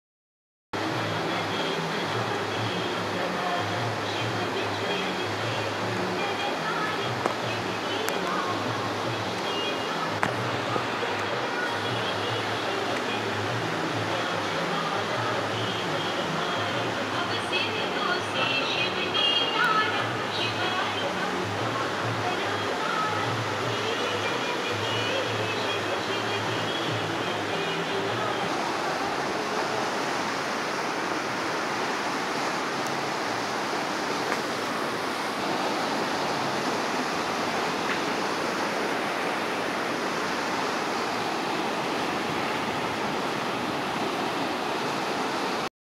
devprayag sangam part 2...
Devprayag (Deva prayāga) is a town and a nagar panchayat (municipality) in Tehri Garhwal district in the state of Uttarakhand, India, and is the final one of the Panch Prayag (five confluences) of Alaknanda River where Alaknanda meets the Bhagirathi river and both rivers thereafter flow on as the Ganges river or Ganga.😘